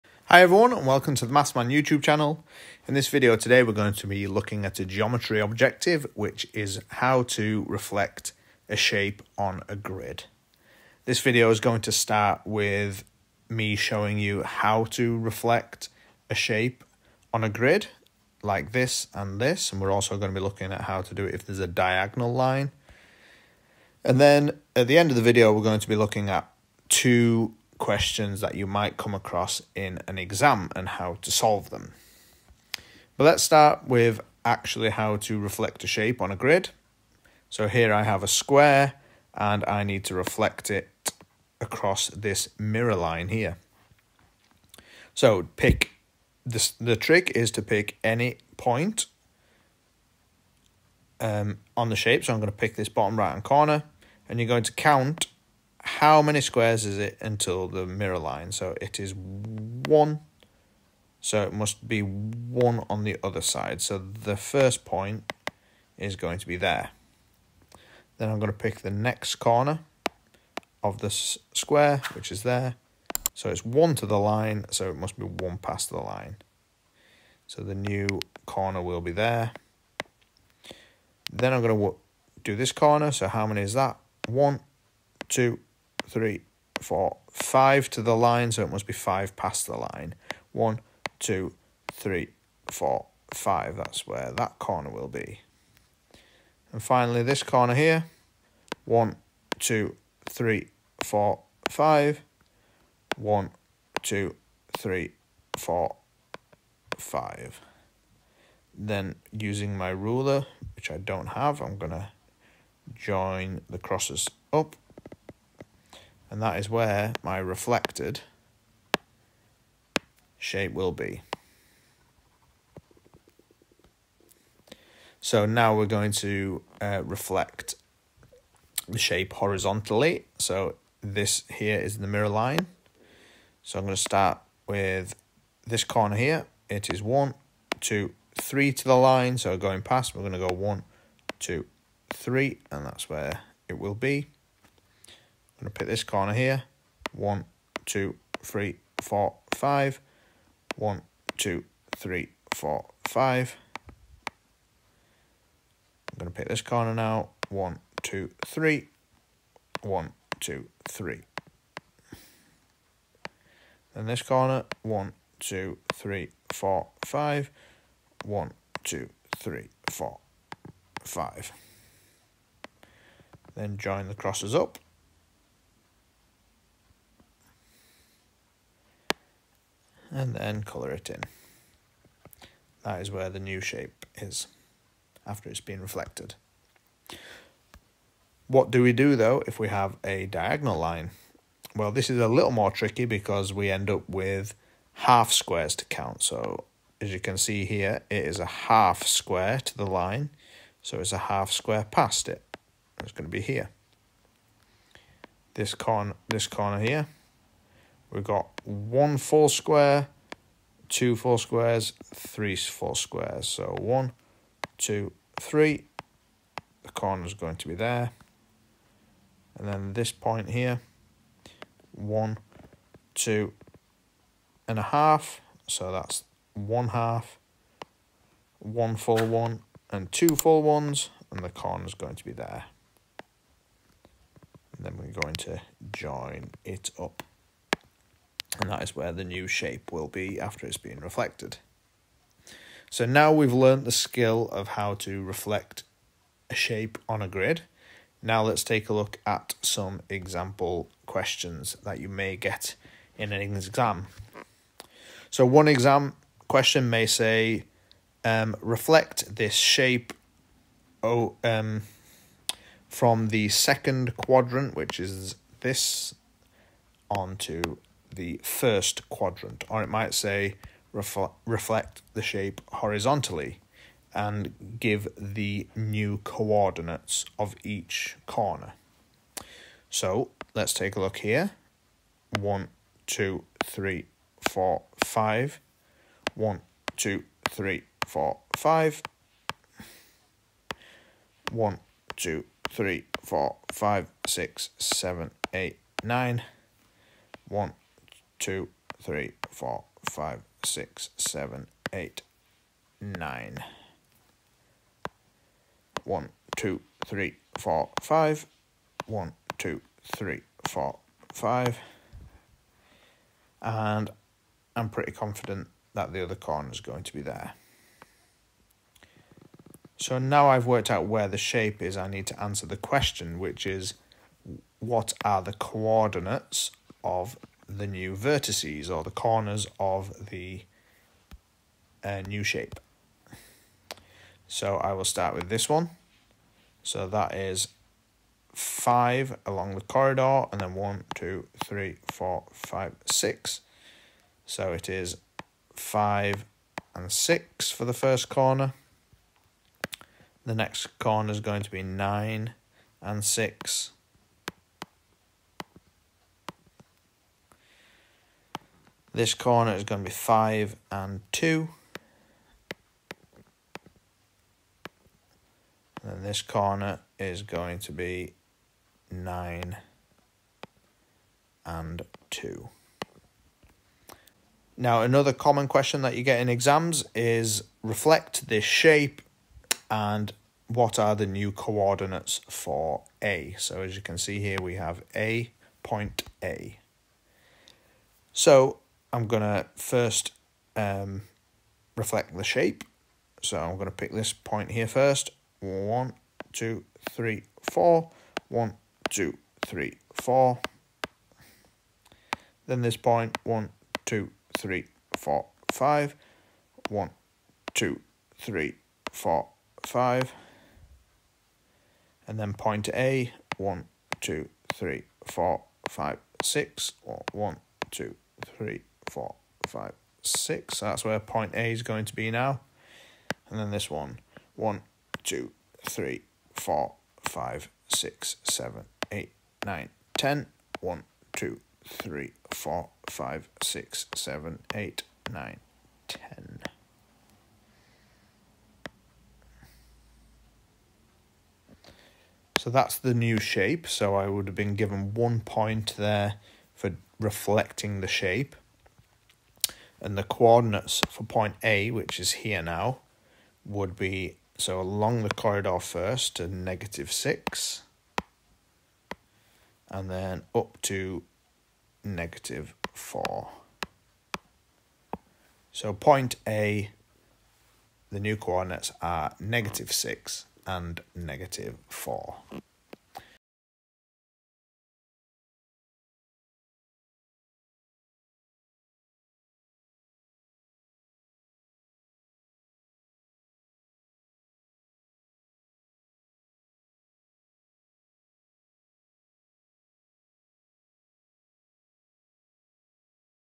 Reflecting shapes on a coordinate grid and across mirror lines | Geometry
Welcome to the  @TheMathsManuk  YouTube Channel! I am a fully qualified UK teacher with a passion for teaching maths!

In this video, I demonstrate how to reflect shapes across a mirror line and on a coordinate grid. This is a very common question which is featured in almost every Key Stage 2 Maths exam.

If you found this video helpful, then you may want to check out the geometry playlist from my channel with other related videos.